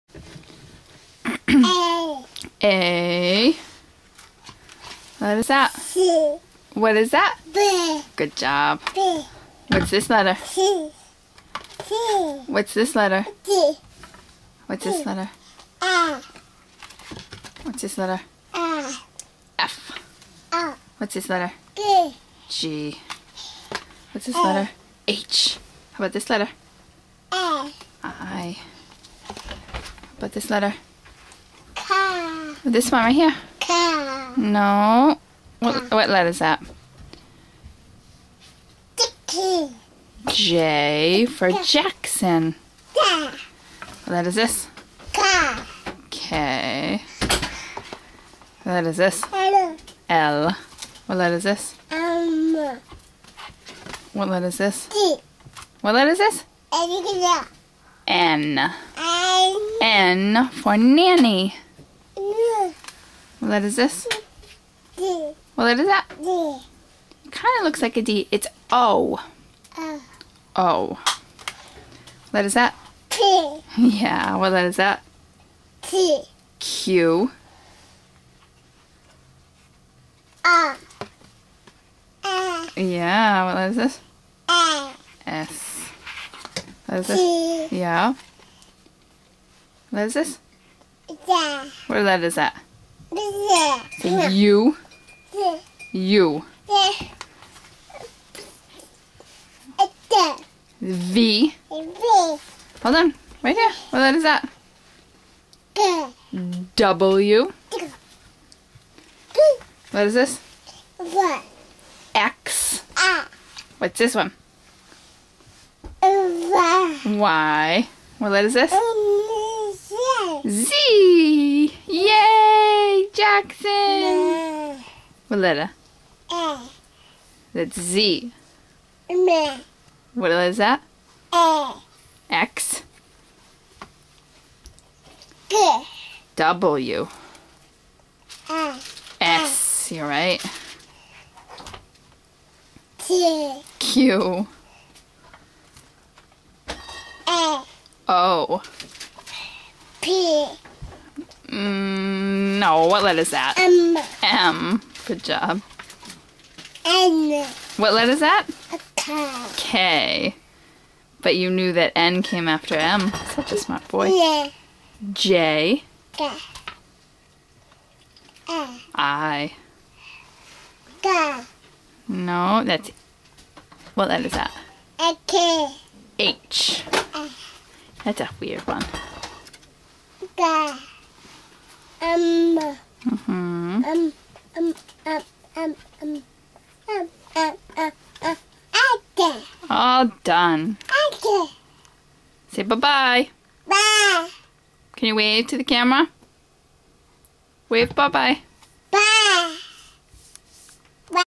Baby Recognizing Letters
Just under 16 months and he started opening his book and pointing out letters! He just keeps amazing me!